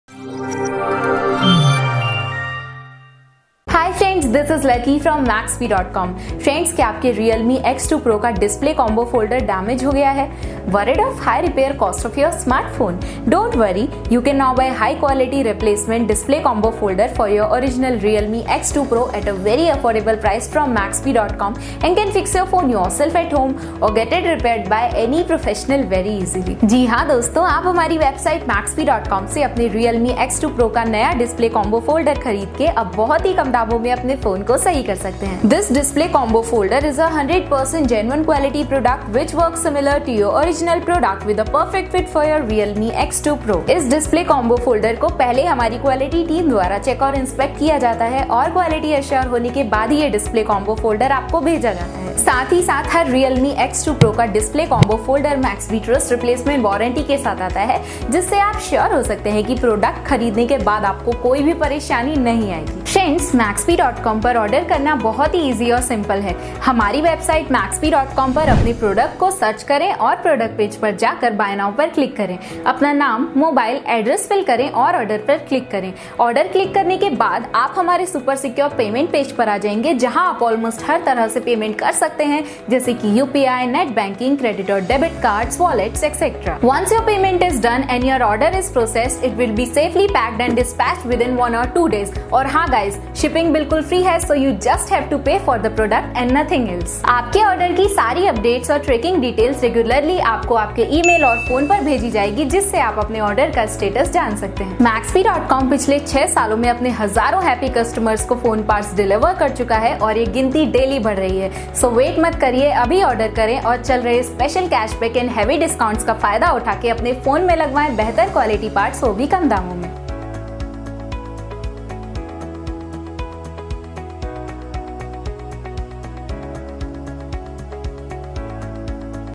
Buy Realme X2 Pro Display Combo Folder, Free Delivery High Quality Best Price Maxbhi
Got broken display in your Realme X2 Pro? Buy the high quality complete display combo for Realme X2 Pro and make your phone like new one.

The replacement combo screen for Realme X2 Pro comes with replacement warranty and the shipping is done in secured packing to make sure you get the product in perfect shape.